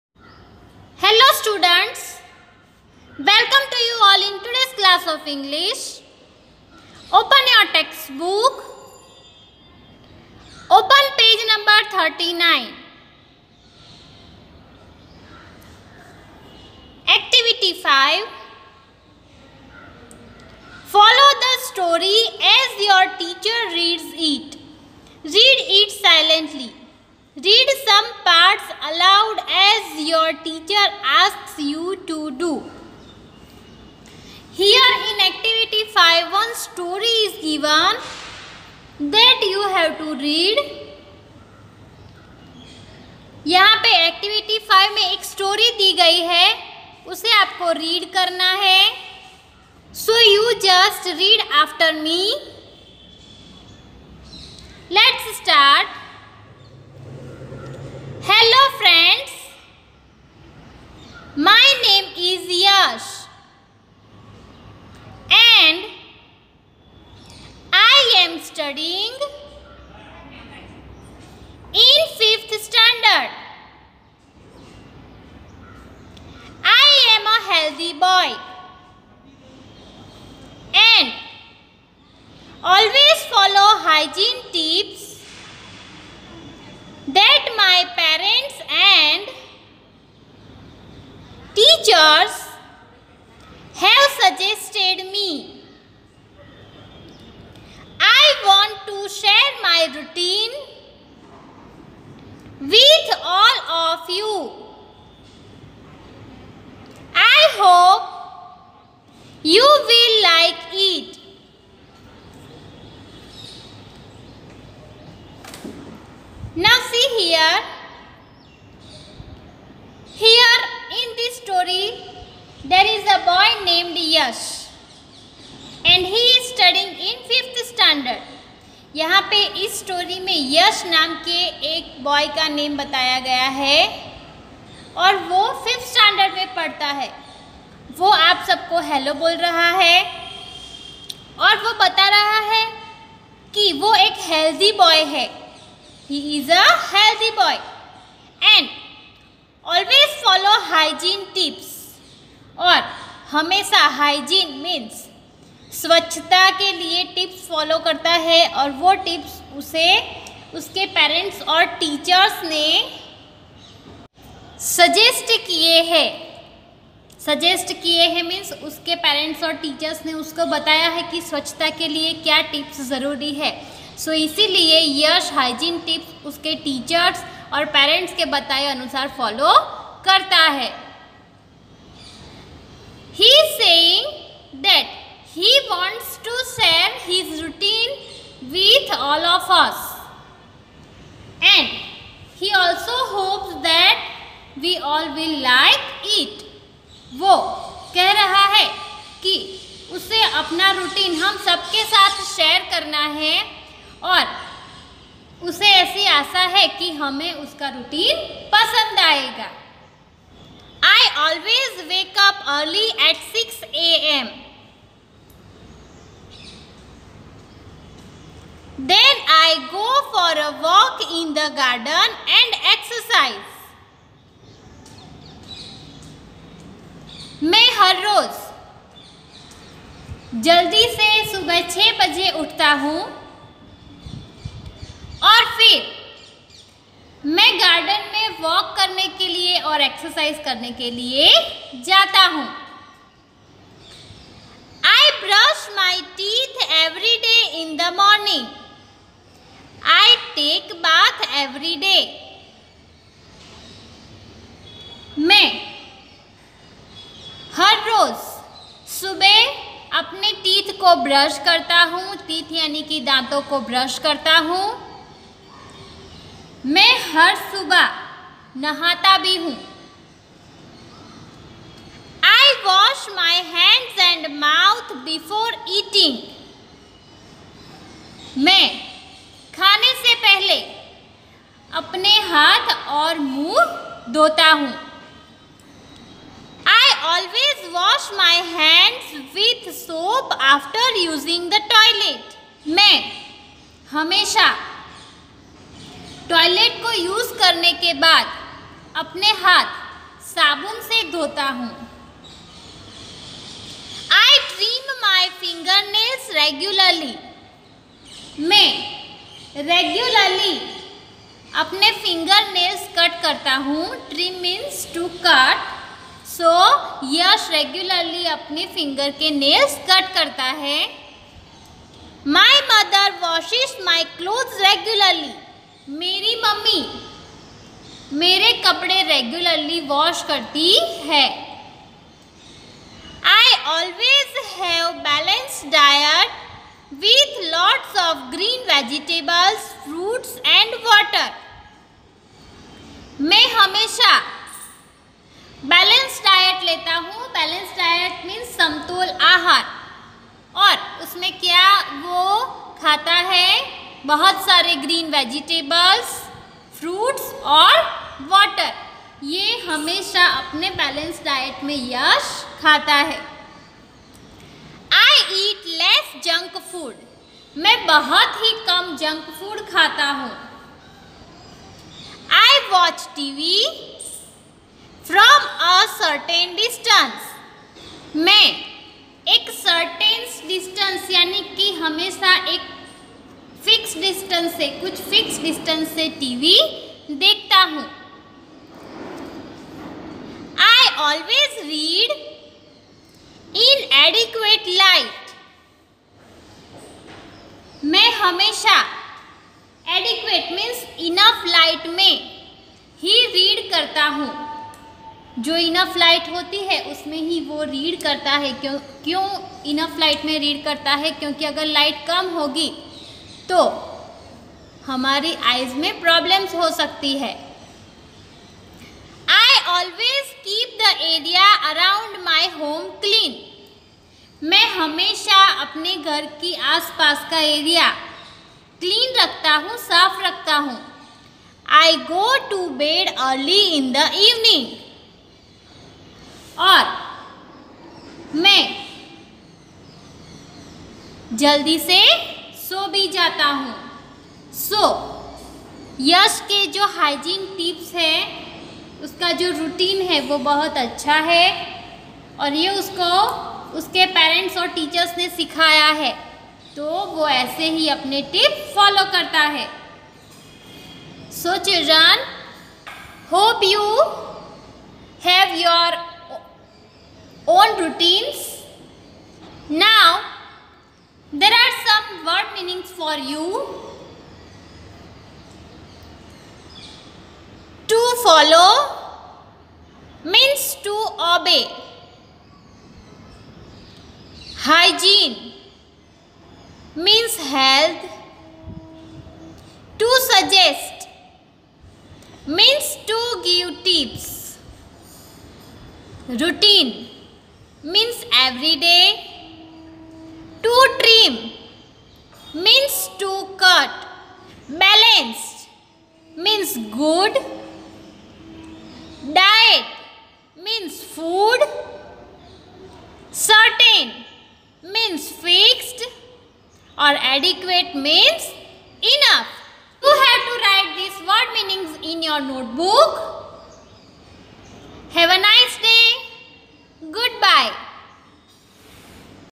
Std.4|English|Unit-5|How do you feel?|Activity-5|Good habits|Health tips|By Krishna Panchal
Tutor- Krishna Panchal
Editor- Jignesh Panchal
Unit-5
How do you feel?
Activity-5
Gcert
NCERT